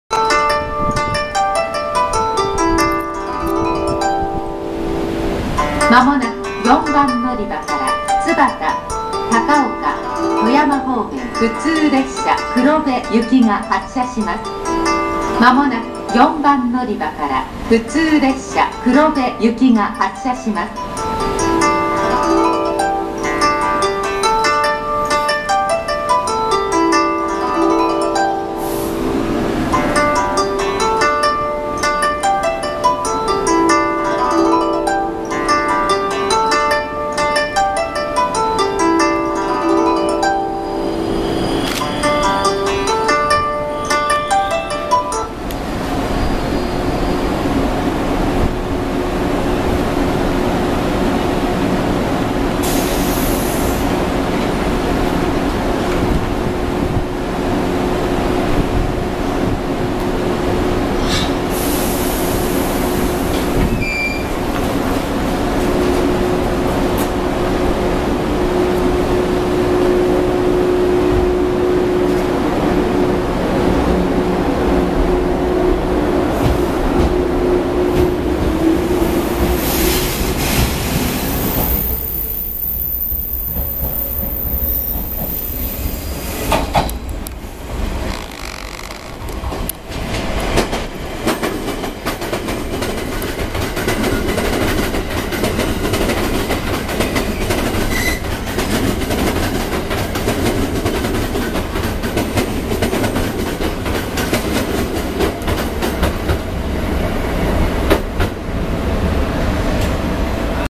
北陸線 413系B04編成 黒部行き 金沢駅発車
2013/08/06撮影
真っ青のクハ455組み込み413系という変態編成B04が金沢駅を発車します。
※カメラの不具合で、おかしくなる部分があります・・・